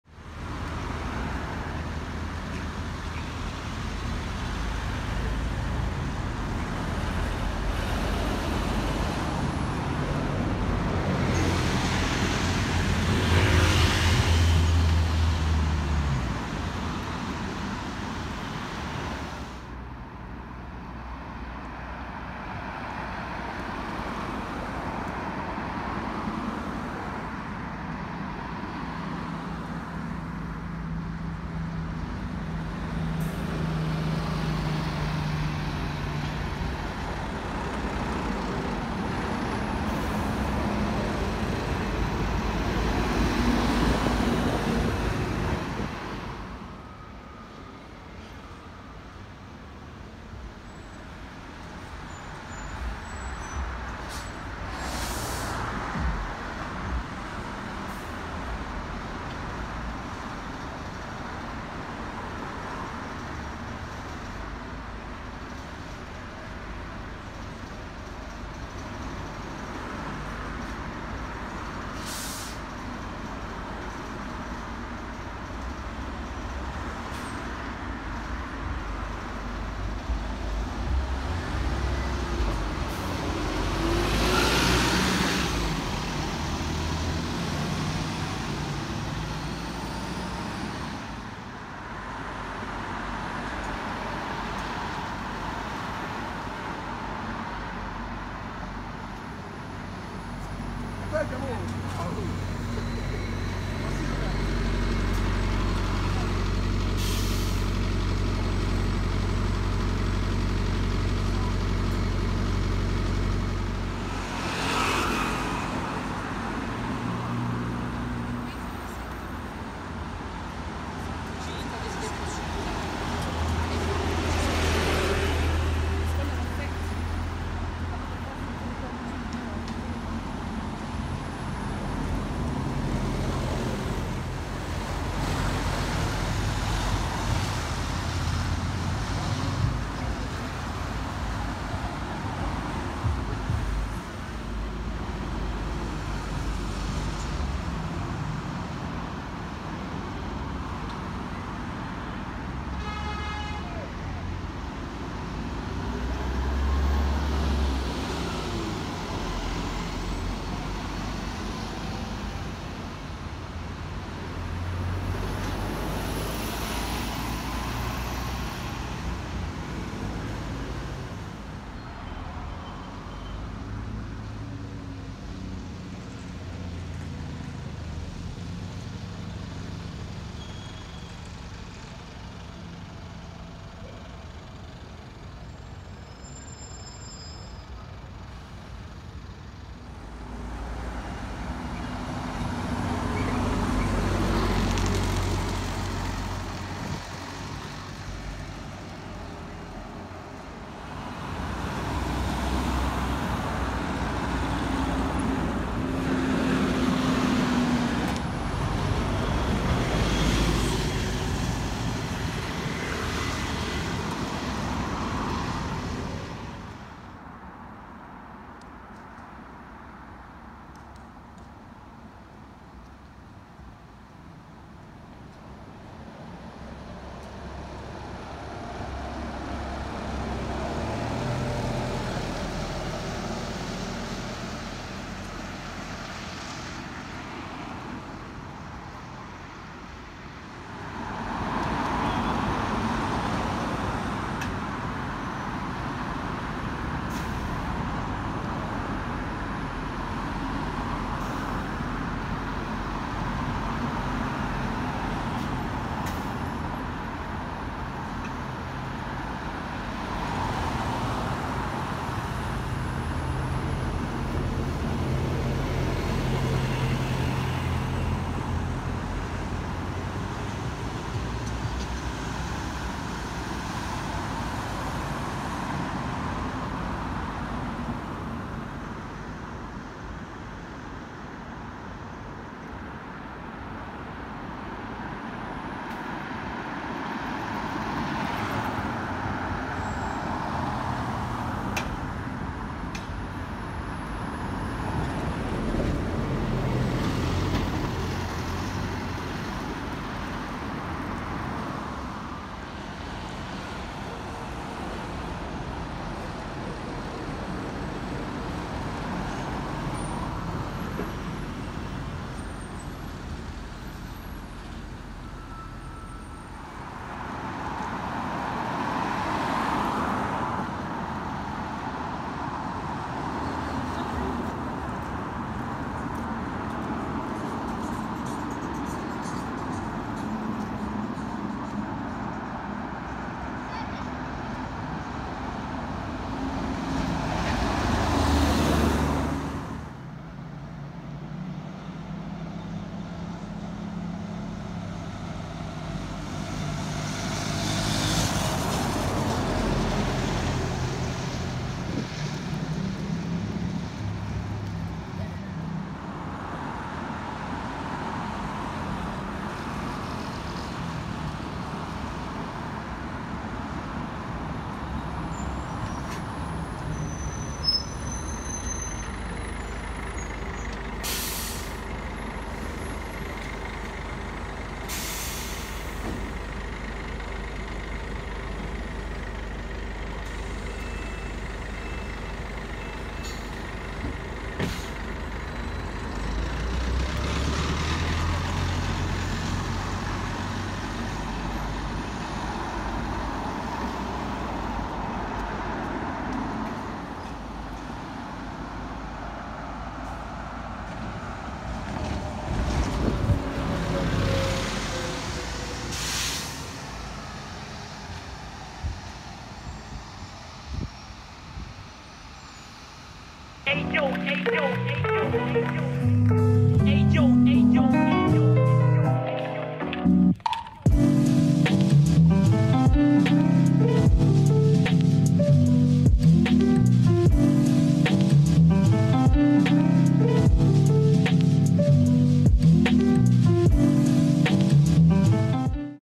London Buses @Manor House / Finsbury Park / Seven Sisters Road
Back to night in North London to catch London buses in the areas of Finsbury Park, and Manor House.

The Piccadilly line had no services between Cockfosters, and King's Cross St Pancras, and railway replacement buses did services between those stations.